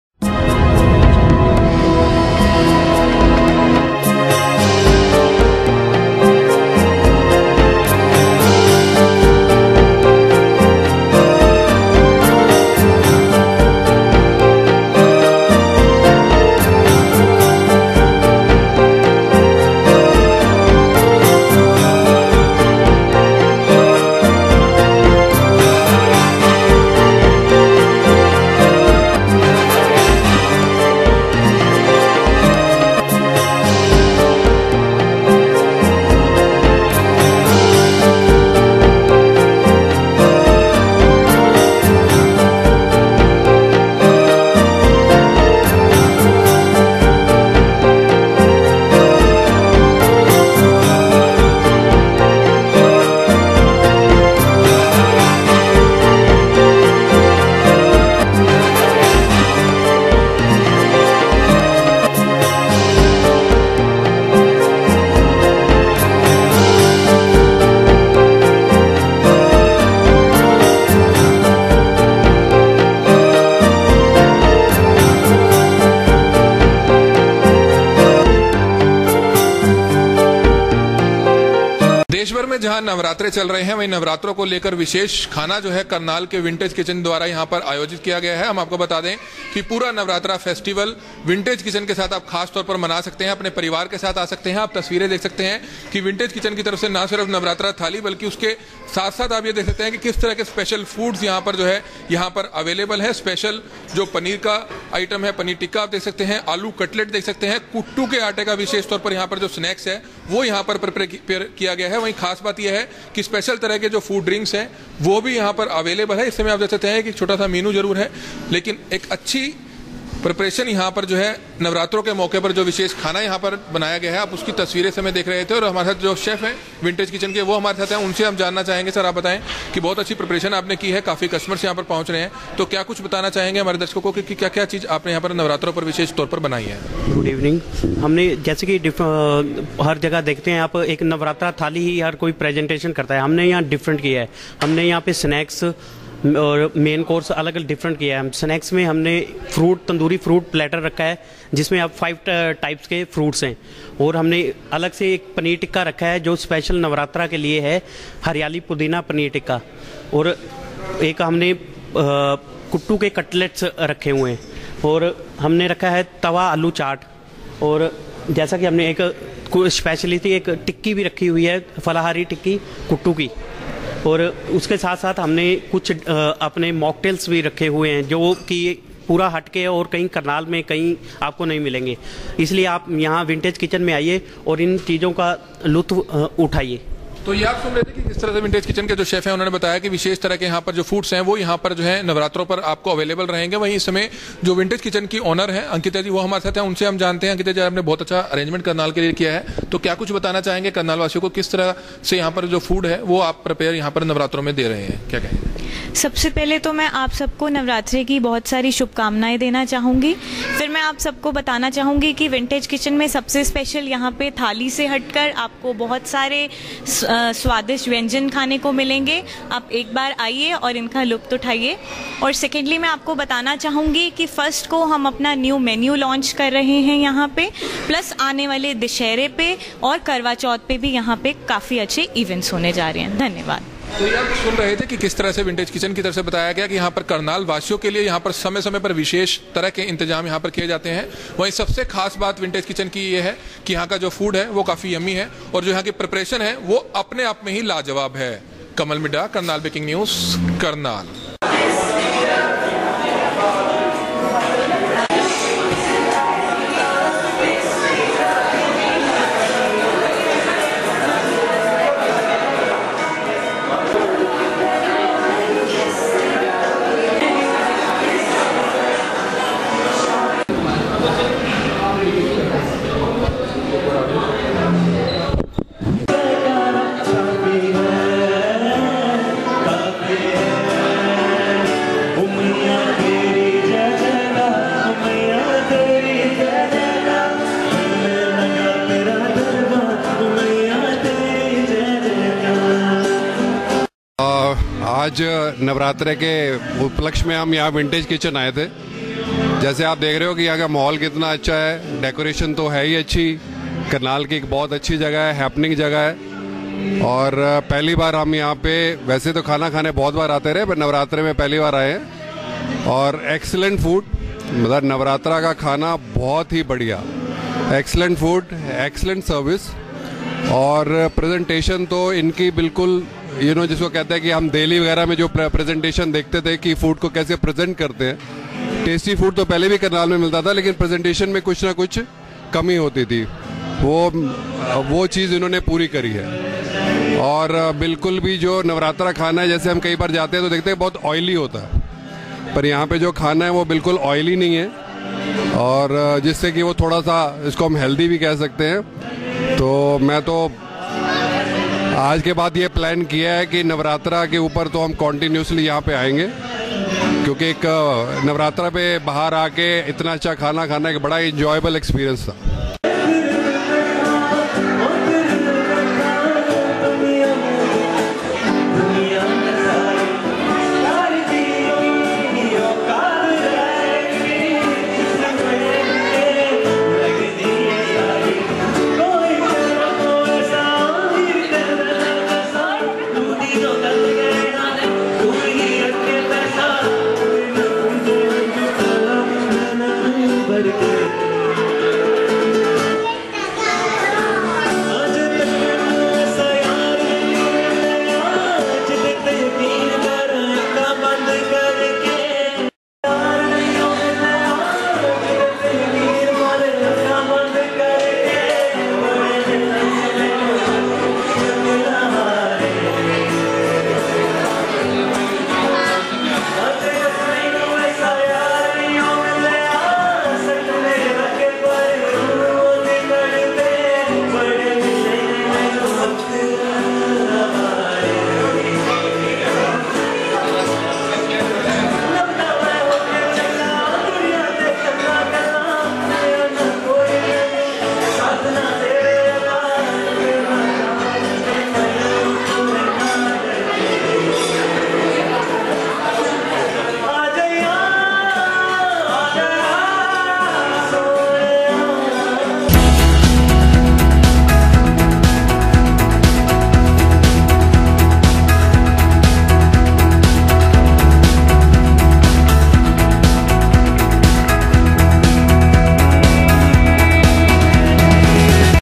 Karnal Vintage Kitchen Navratra Food & Live Band Music Watch & Share
Karnal Vintage Kitchen Navratra Food & Live Band Music Watch & Share - टेस्टी फ़ूड साथ में लाईव बैंड म्यूसिक सिर्फ Vintage Kitchen करनाल पर , वही नवरात्रों पर विशेष खाने का इंतजाम अपने ग्राहकों के लिए विंटेज किचन में किया गया है ,हेल्थी एंड टेस्टी फूड ओर एक्सीलेंट प्रेपरेशन के लिए जाना जाता है Vintage Kitchen ,देखें लाईव वीडियो - Watch & Share